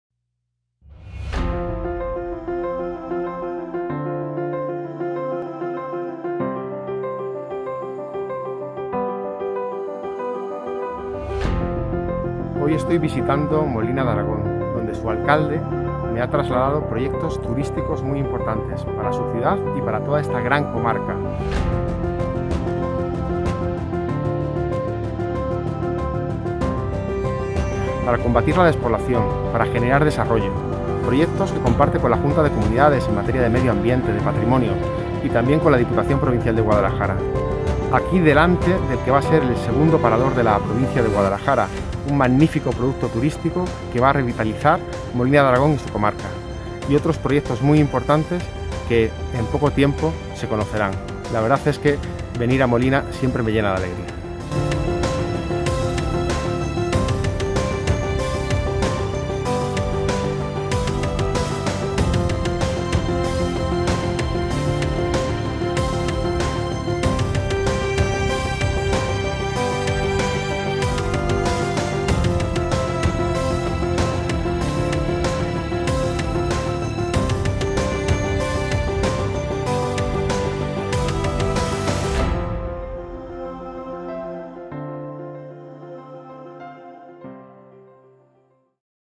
Visita de Pablo Bellido a Molina de Aragón (Guadalajara)
El presidente de las Cortes de Castilla-La Mancha, Pablo Bellido, ha visitado el municipio guadalajareño de Molina de Aragón de la mano de su alcalde, Francisco Javier Montes Moreno, para conocer distintos proyectos turísticos previstos en la localidad y su entorno.